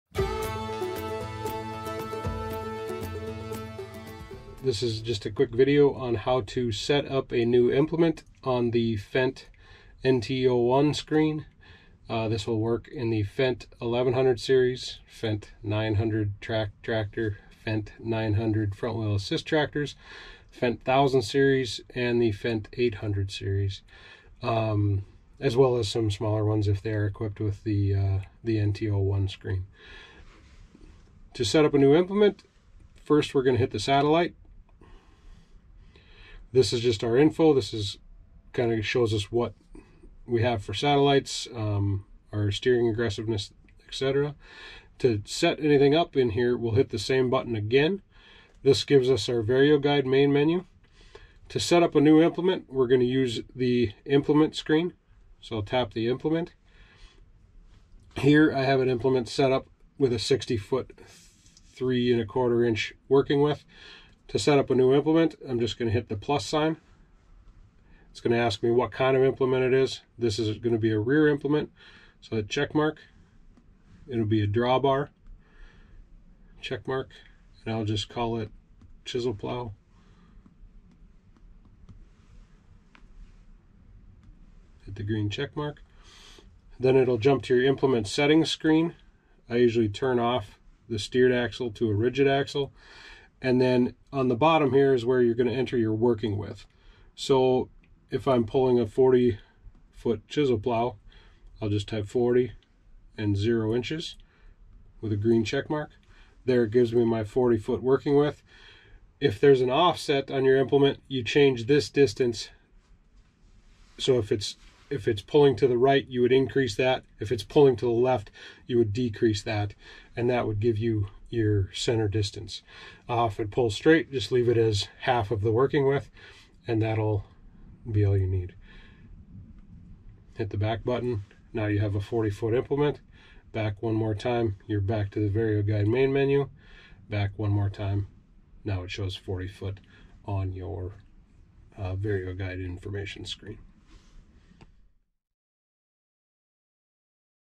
Setting Up A New Implement on the Fendt VarioTerminal NT01 Monitor
Nate from Butler demonstrates how to set up a new implement on the Fendt VarioTerminal NT01 screen. These instructions apply to the NT01 screens in Fendt 800 Series, Fendt 900 Series, Fendt 1000 Series, Fendt 900MT Tracked Series and Fendt 1100MT Tracked Series tractors.